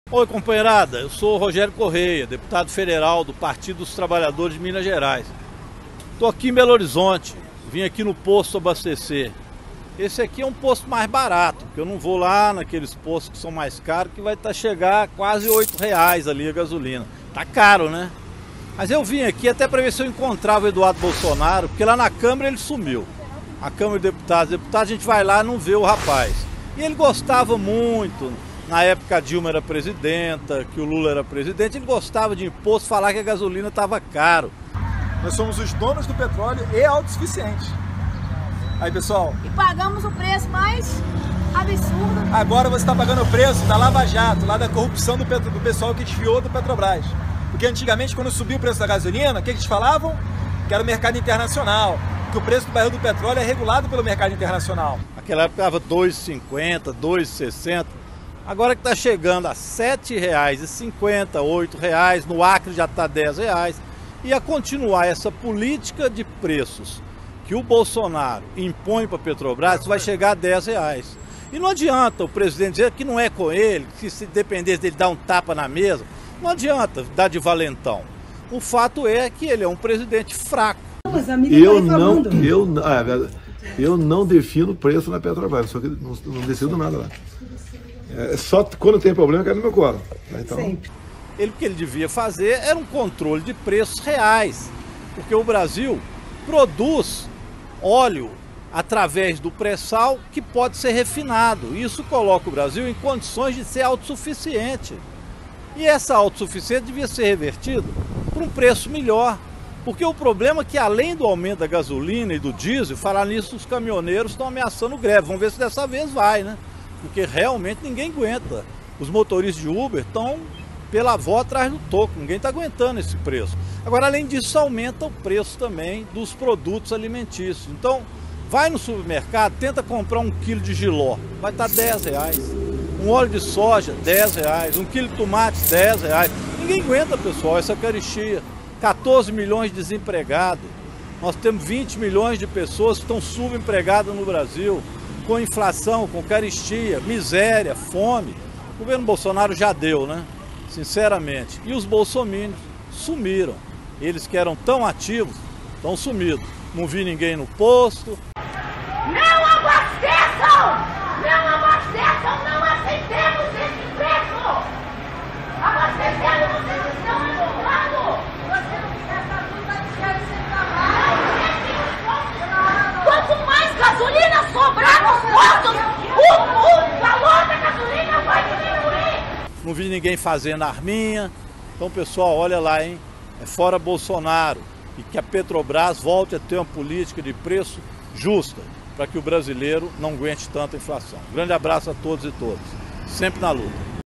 O deputado Rogério Correia foi a um posto de gasolina em BH e encontrou o preço nas alturas!
DEPUTADO VAI A POSTO DE GASOLINA E PROVOCA: "UAI, CADÊ O EDUARDO BOLSONARO PROTESTANDO?"
Vale a pena assistir ao vídeo: o deputado federal Rogério Correia (PT-MG) foi a um posto de gasolina em BH e, claro, encontrou o preço do litro nas alturas (e olha que nem era o mais caro na cidade...). Ele aproveitou e provocou Eduardo Bolsonaro, também deputado e filho do presidente, que adorava visitar postos para protestar contra o que dizia ser "preço abusivo" da gasolina (e que não passava dos R$ 3,50 e olhe lá...). Confira e, se gostar e achar importante, por favor compartilhe. Obrigado